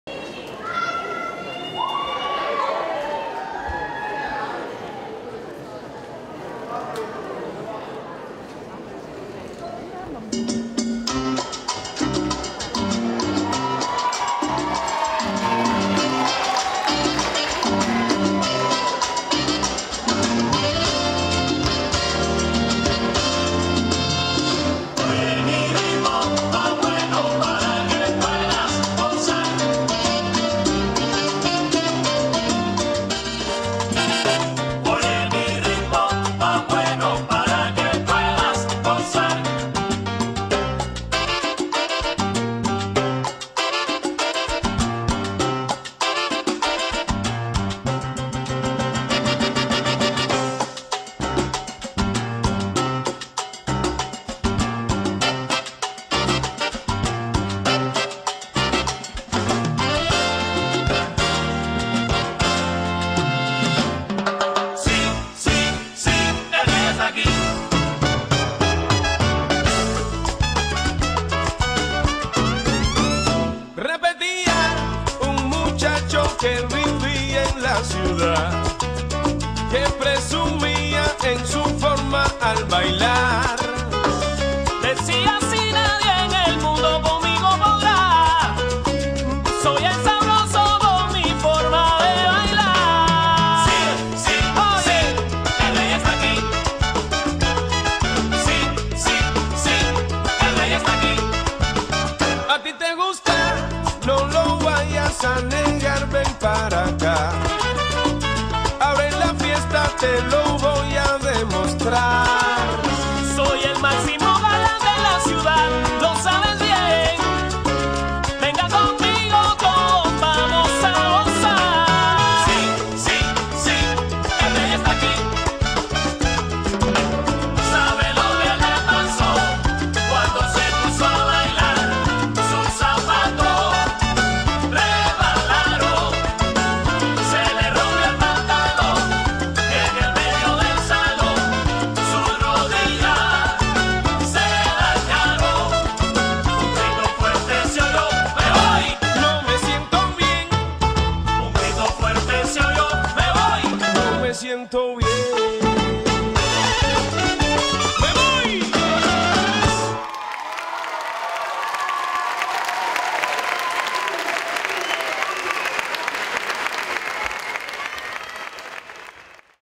Ritmo mas bueno ...si si!. L.Parrini, L.Fantacci- MAX BALLET ACADEMY
tratto dallo spettacolo al Saschall giugno 2010.